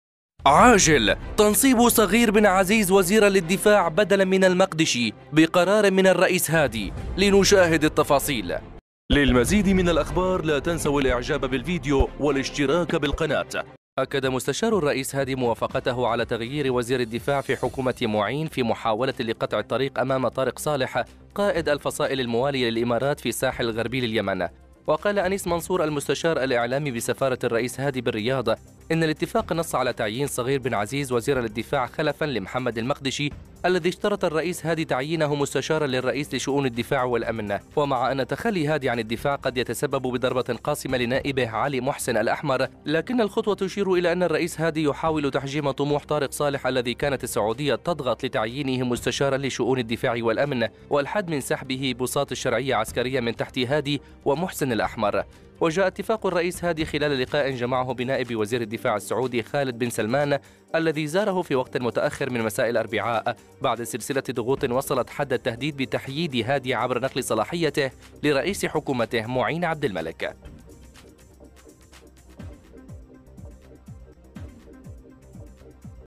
عاجل💥تنصيب صغير بن عزيز وزيرا للدفاع بدلا عن المقدشي بقرار من الرئيس هادي. !!
أكد مستشار الرئيس هادي، موافقته على تغيير وزير الدفاع في حكومة معين في محاولة  لقطع الطريق أمام طارق صالح، قائد الفصائل الموالية للإمارات في الساحل الغربي لليمن.
وقال أنيس منصور، المستشار الإعلامي  بسفارة الرئيس هادي بالرياض إن الاتفاق نص على تعيين صغير بن عزيز وزيرا للدفاع خلفا لمحمد المقدشي الذي اشترط الرئيس هادي تعيينه مستشارا للرئيس لشؤون الدفاع والأمن.
ومع أن تخلي هادي عن الدفاع قد يتسبب بضربة قاصمة لنائبه علي محسن الاحمر، لكن الخطوة تشير إلى أن الرئيس هادي يحاول تحجيم طموح طارق صالح الذي كانت السعودية تضغط لتعينه مستشار لشؤون الدفاع والأمن، والحد من سحبه بساط “الشرعية” عسكريا من تحت هادي ومحسن الاحمر .
وجاء اتفاق الرئيس هادي خلال لقاء جمعه بنائب وزير الدفاع السعودي، خالد بن سلمان، الذي زاره في وقت متأخر من مساء الأربعاء، بعد سلسلة ضغوط وصلت حد التهديد بتحييد هادي عبر نقل صلاحيته لرئيس حكومتة معين عبدالملك .